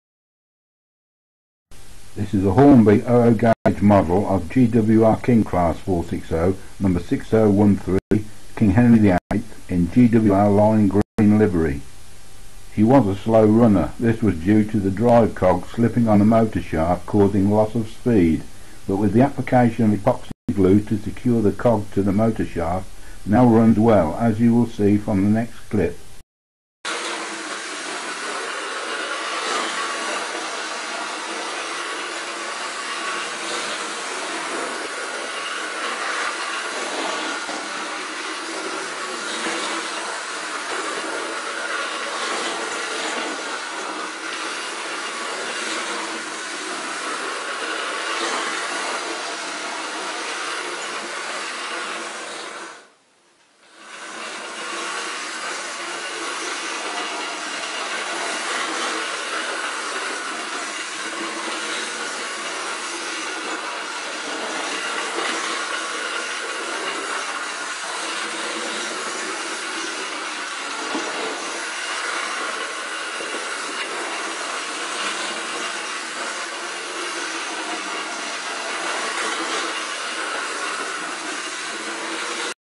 Hornby OO Gauge model. GWR King Class. 4-6-0. No 6013 King Henry VIII
Repair details and running on my test tracks